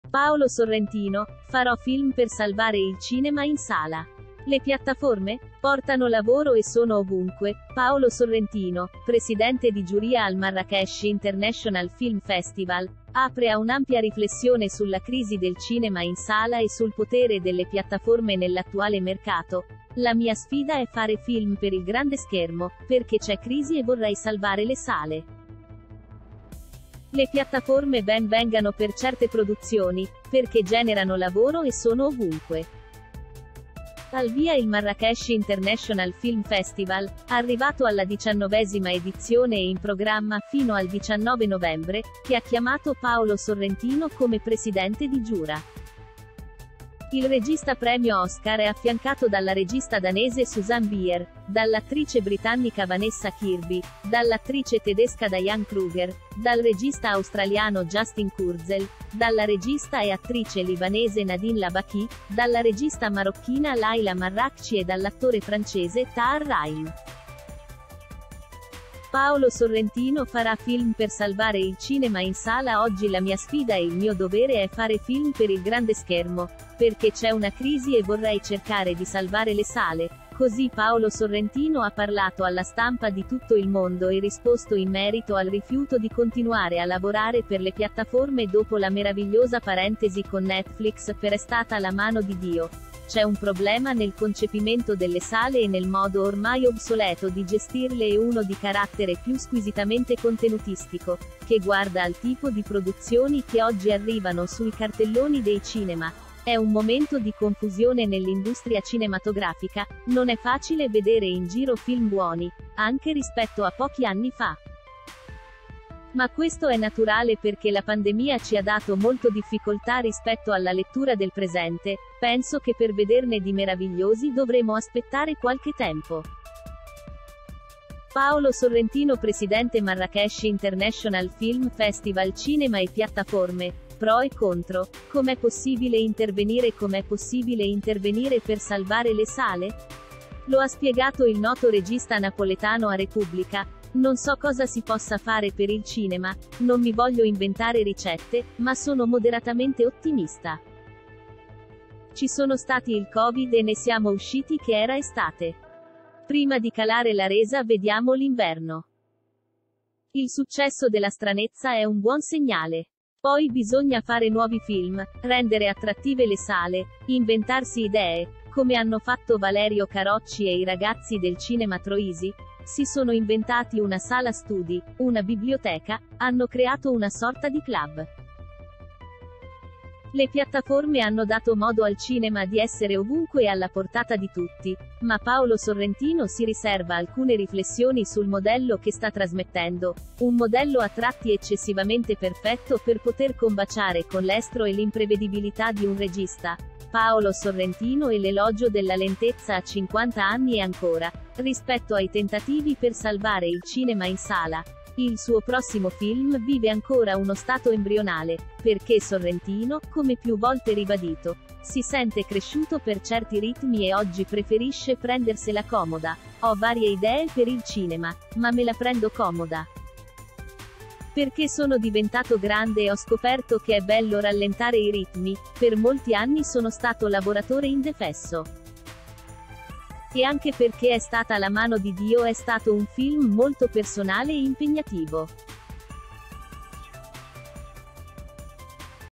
Paolo Sorrentino: “Farò film per salvare il cinema in sala. Le piattaforme? Portano lavoro e sono ov
Paolo Sorrentino: “Farò film per salvare il cinema in sala. Le piattaforme? Portano lavoro e sono ovunque”   Paolo Sorrentino, presidente di giuria al Marrakech International Film Festival, apre a un’ampia riflessione sulla crisi del cinema in sala e sul potere delle piattaforme nell’attuale mercato: “La mia sfida è fare film per il grande schermo, perché c’è crisi e vorrei salvare le sale. Le piattaforme ben vengano per certe produzioni, perché generano lavoro e sono ovunque”.   Al via il Marra